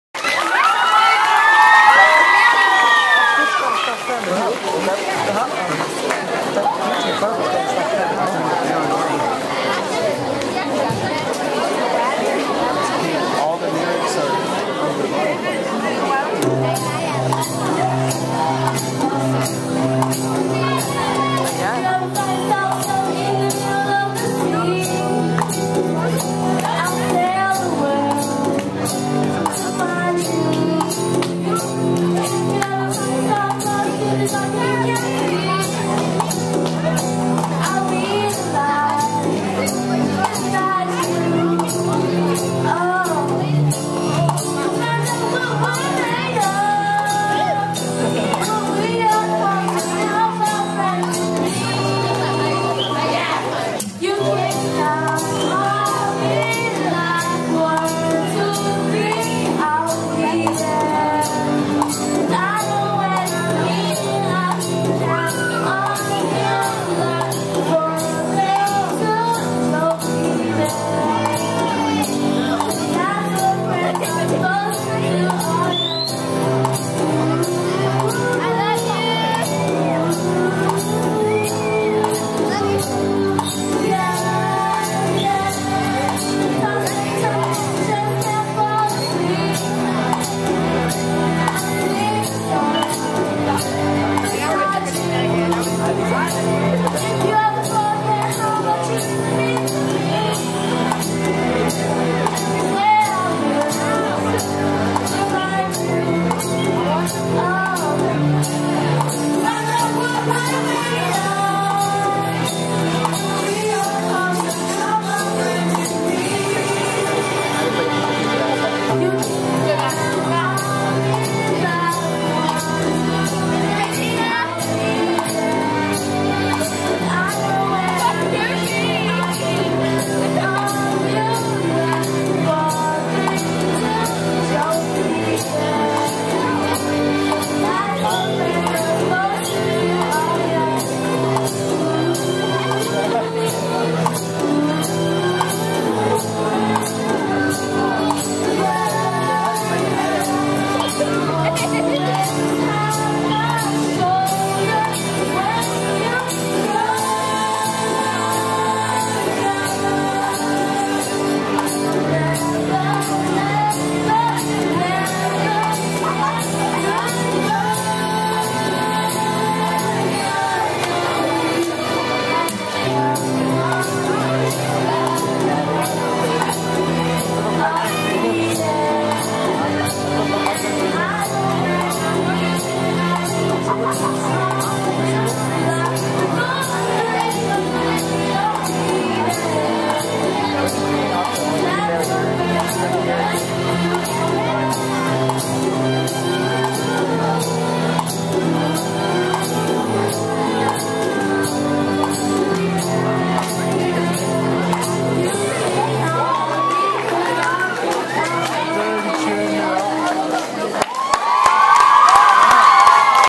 North Park Middle School Talent Show 2011 PART 4
This is part 4 of the North Park Middle School Talent Show. Please no Hate Comments! These students trained hard to be in the talent show.